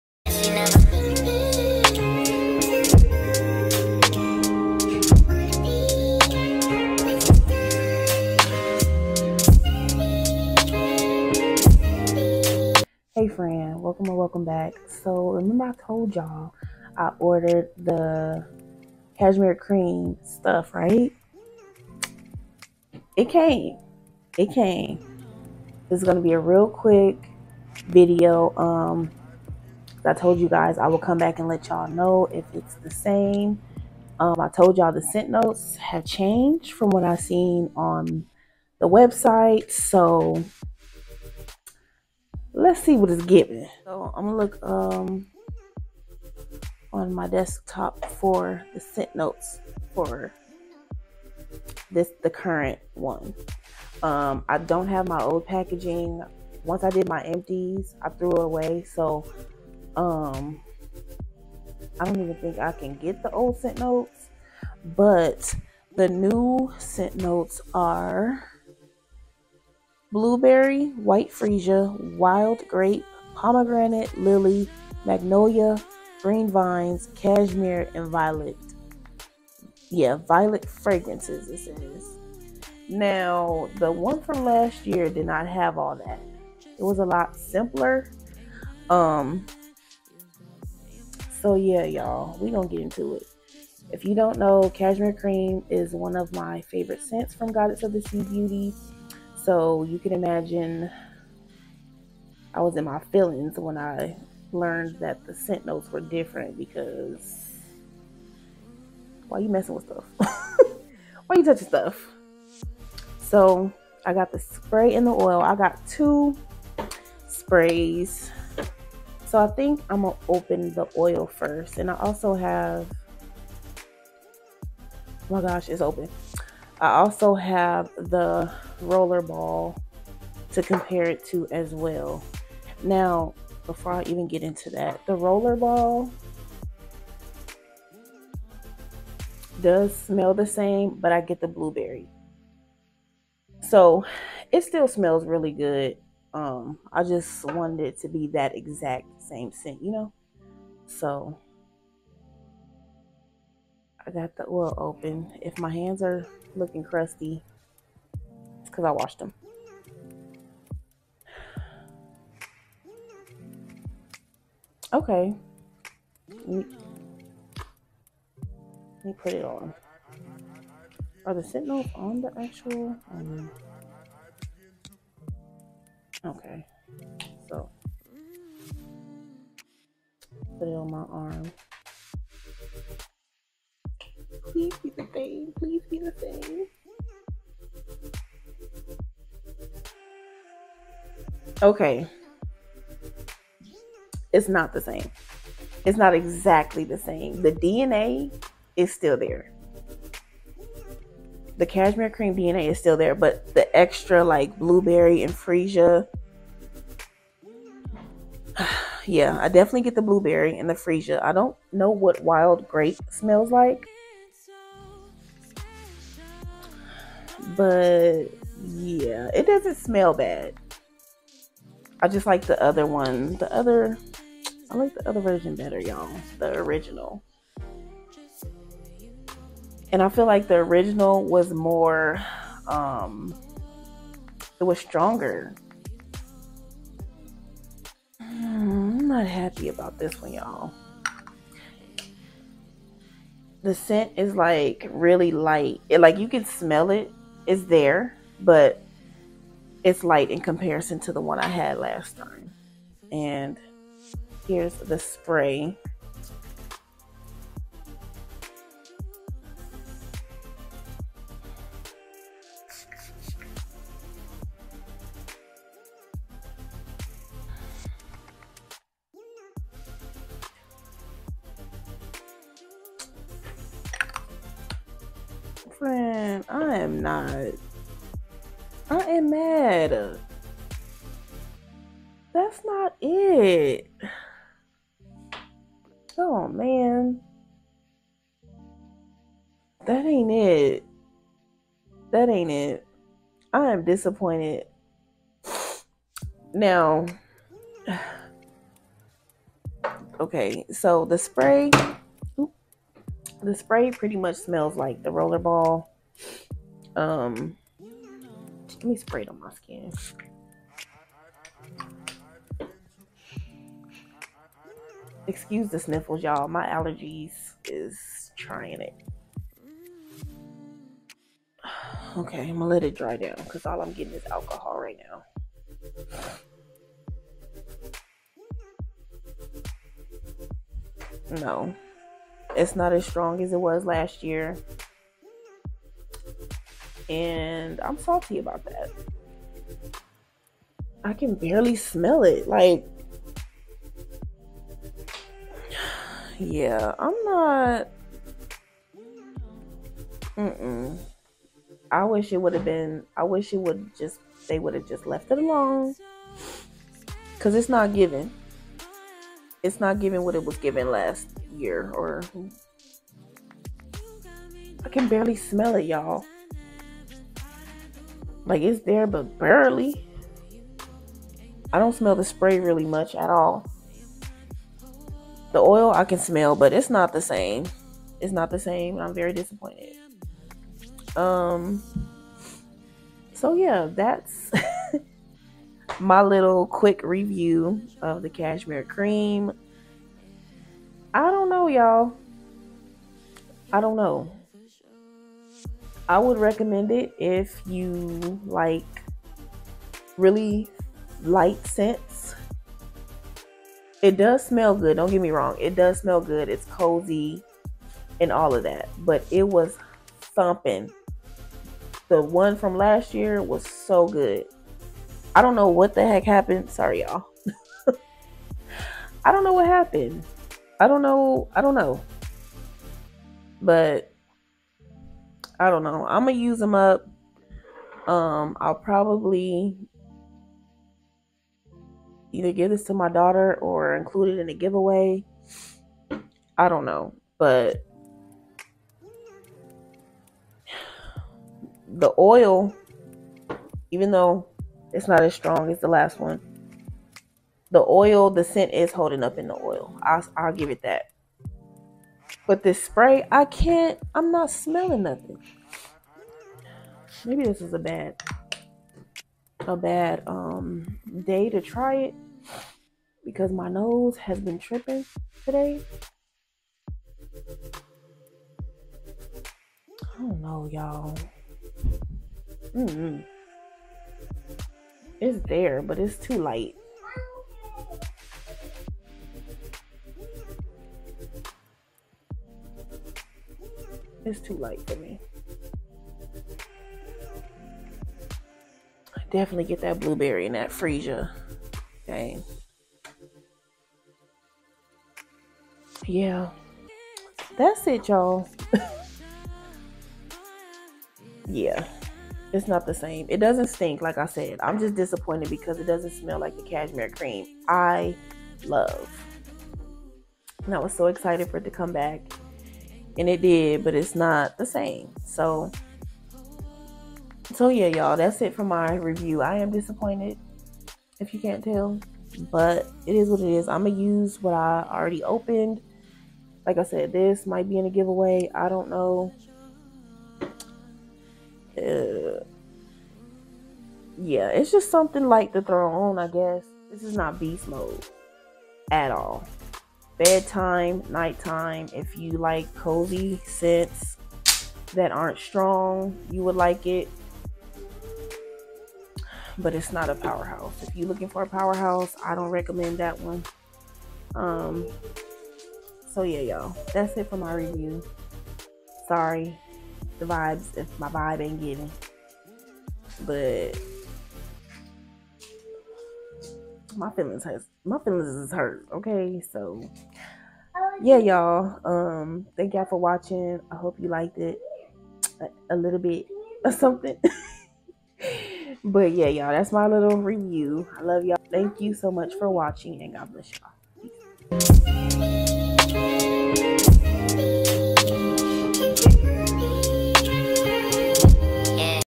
Goddess of the Sea Review| Cashmere Cream… Different scent notes? Is it the same?
Y’all! No words and my feelings are hurt ok lol

Shop Goddess of the Sea Beauty!

- Products Mentioned